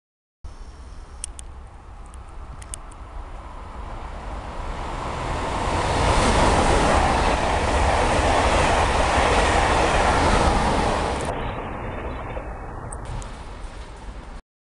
TGV PSE rénové en Atlantique en gare de Libourne!
Un TGV a Libourne sur la voie 2.